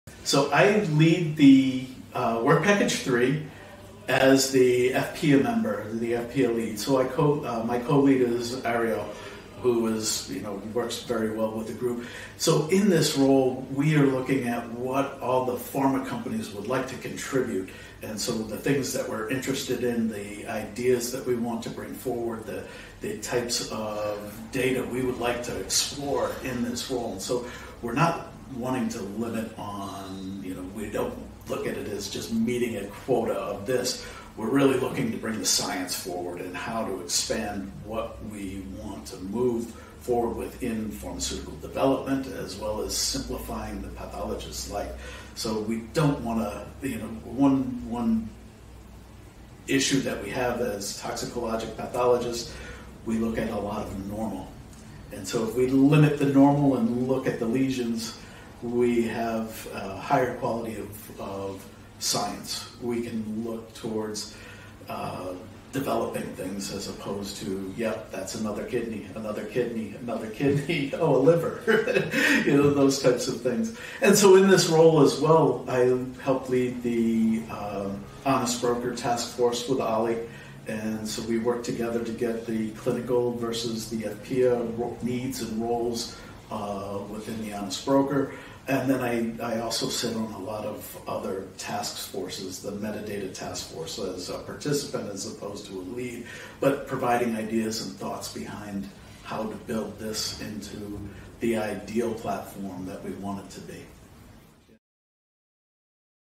Bigpicture: Meet Brian Knight (short version)
Dr. med. vet. Brian Knight is Bigpicture's Lead EFPIA in Work Package 3. In this video, he tells about his several roles in the project. Brian is Senior Research Fellow at Boehringer Ingelheim.